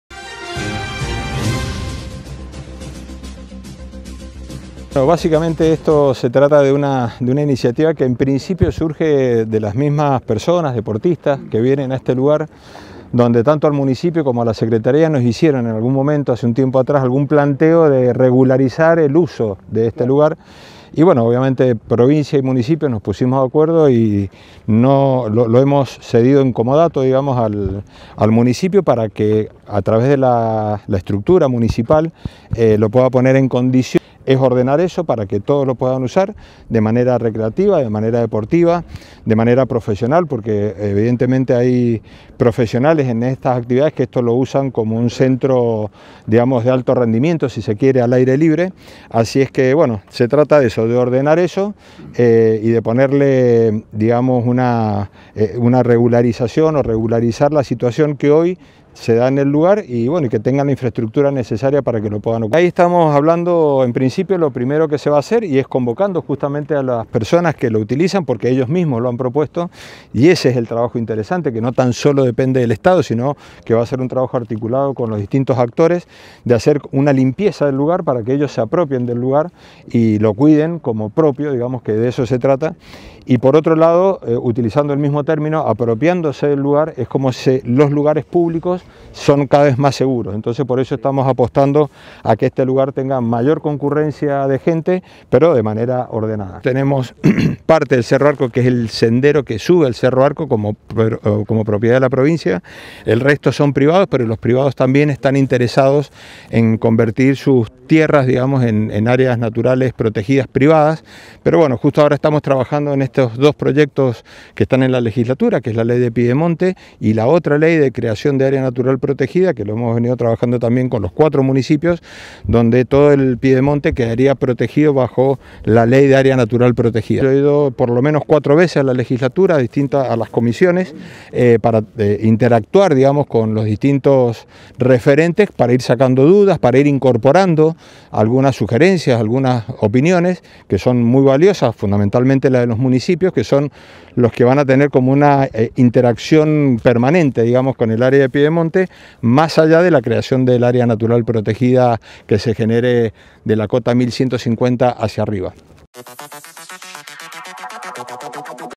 EL DEPORTE DE FIESTA
El Ministro de Ambiente, Humberto Mingorance dialogó con los medios explicando los alcances de este nuevo proyecto encarado desde la Municipalidad de Mendoza y el Ministerio.